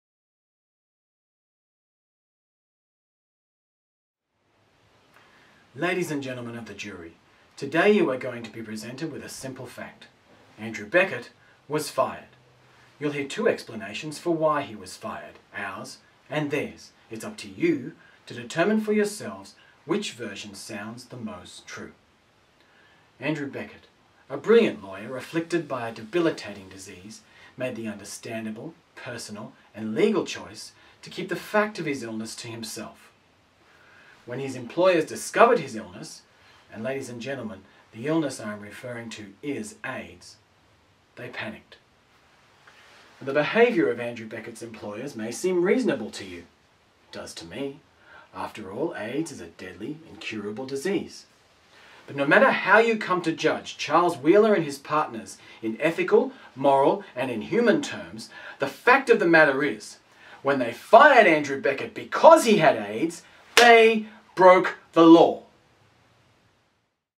Monologue adapted from the film 'Philadelphia', starring Tom Hanks & Denzel Washington
A one minute monologue version of Joe Miller's (played by Denzel Washington) final address to the jury in the film 'Philadelphia' (with an Australian accent).